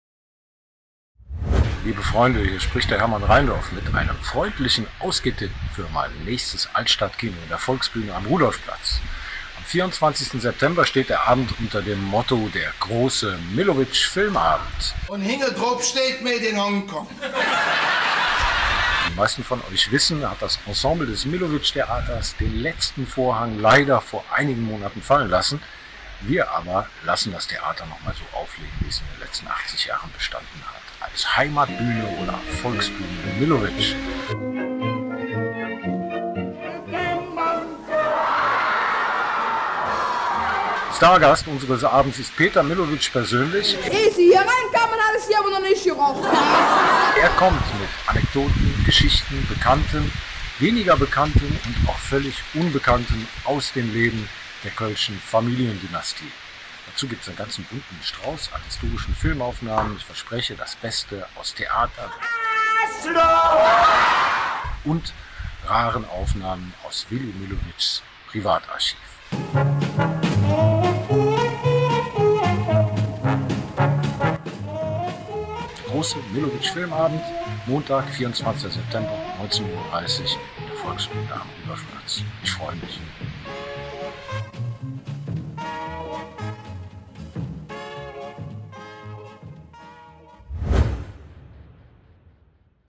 Der große Millowitsch Filmabend mit Peter Millowitsch
Der große Millowitsch Filmaebend findet am Montag, 24. September, 19.30 Uhr in der Volksbühne am Rudolfplatz statt.

Quelle: Hermann Rheindorf